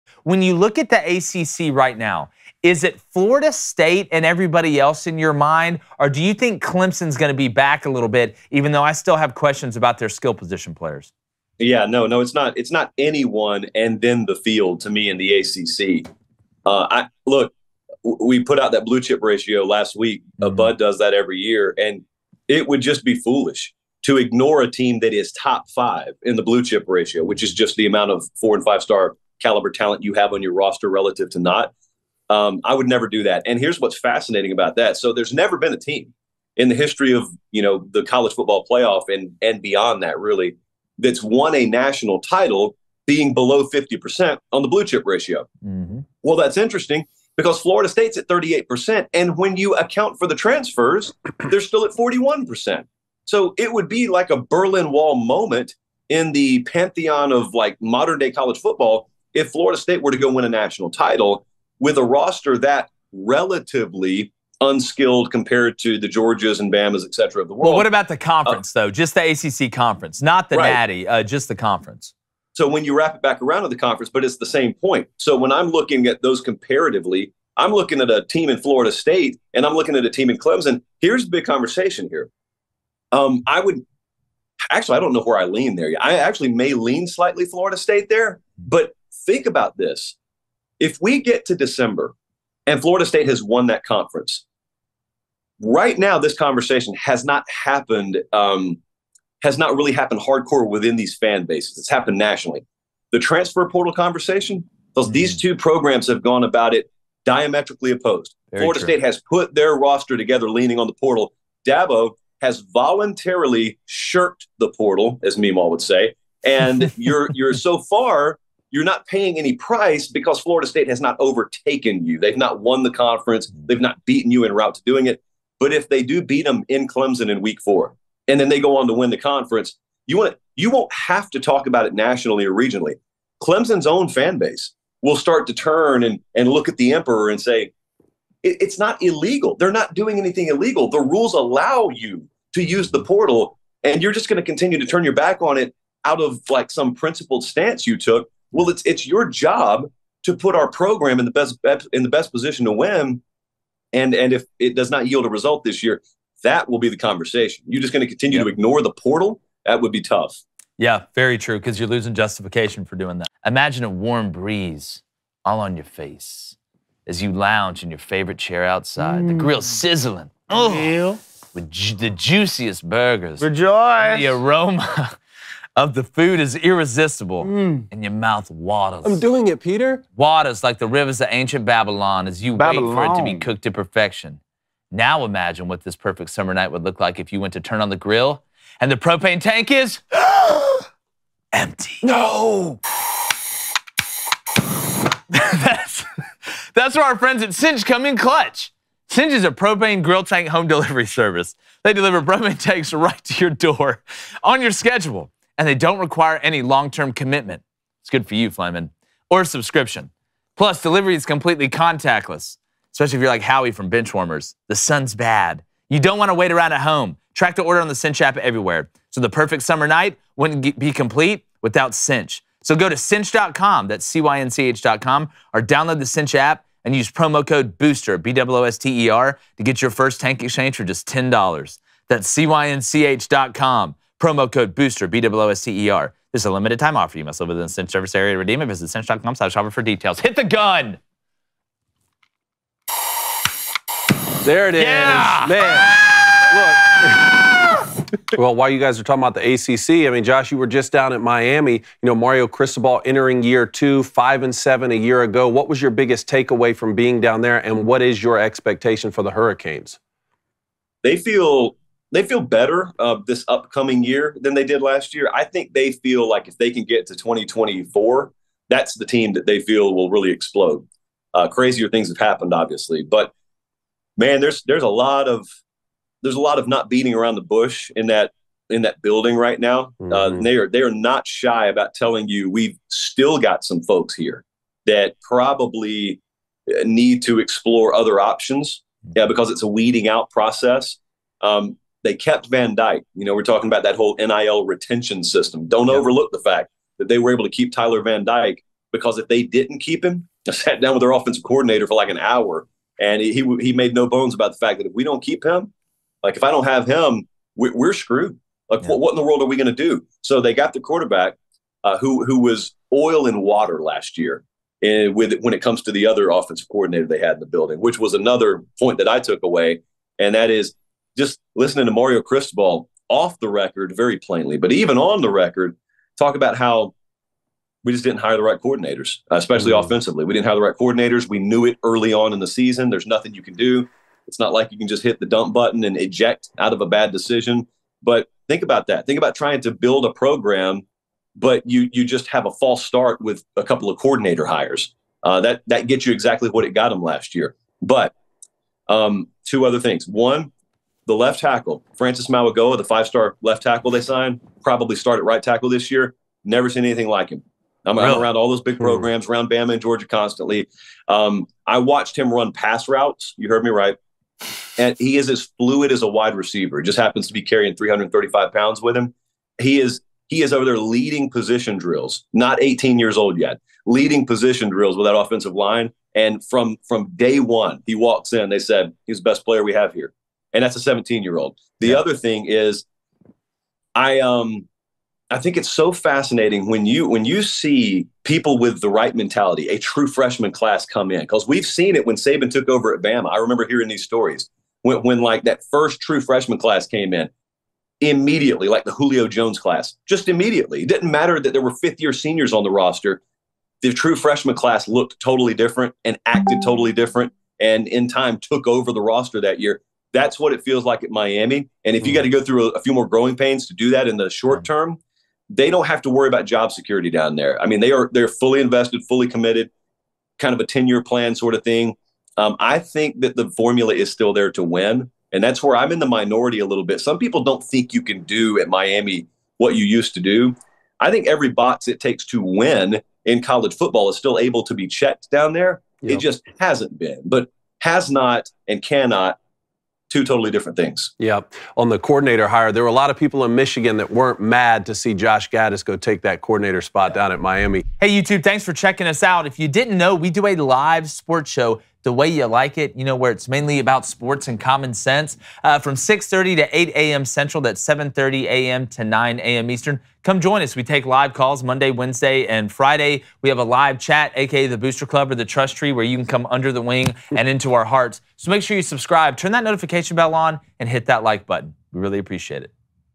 2023 OVERRATED College Football Teams with Josh Pate
Josh Pate joins to talk about the upcoming football season and teams in the ACC that may surprise you in 2023, and teams that may be a little overrated.

(Twitter & Instagram) @CrainCompany
(TikTok) @Crain&Company